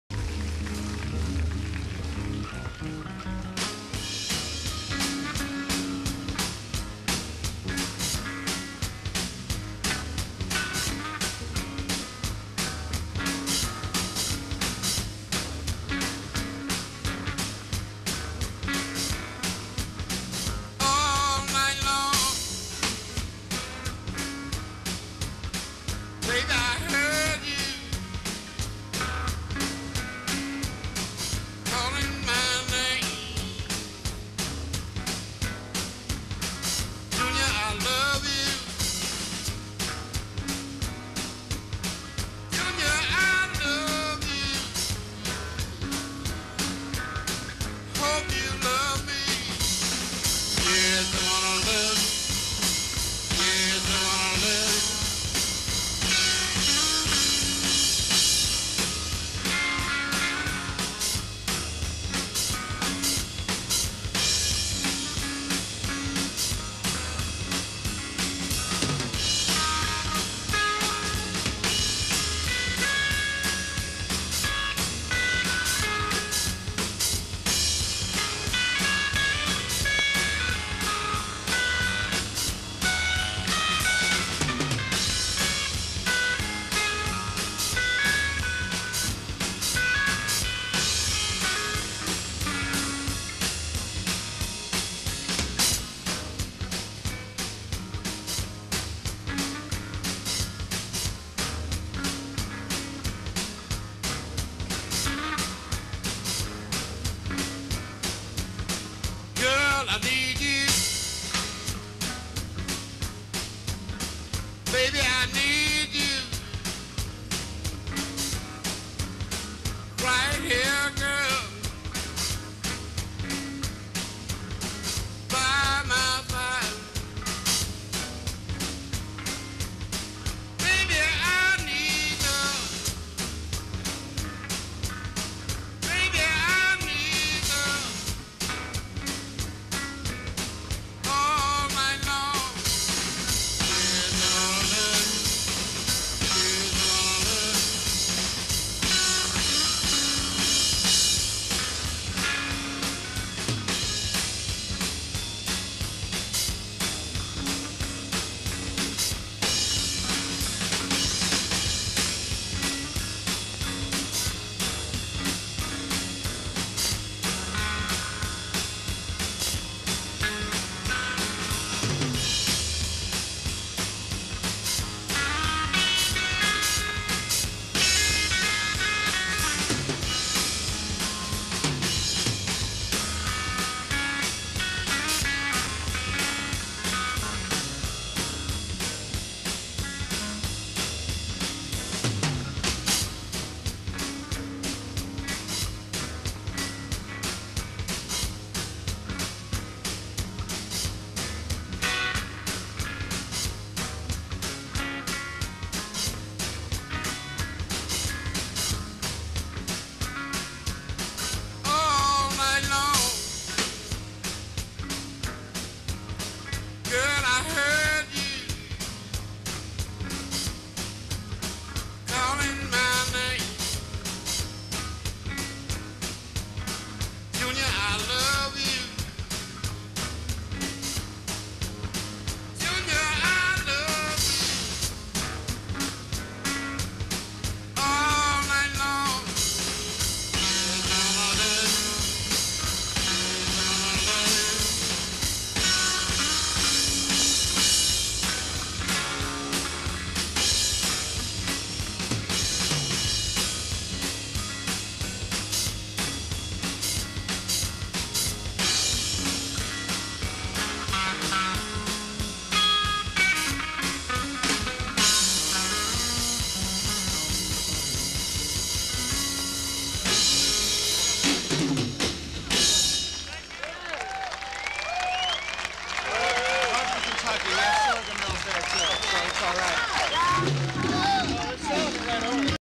Junior Kimbrough - Chicago Blues Festival (1995) Part 2
Junior Kimgrough (vocal & guitar), Lil Dave Thompson (bass) Cedric Burnside (drums) More Blues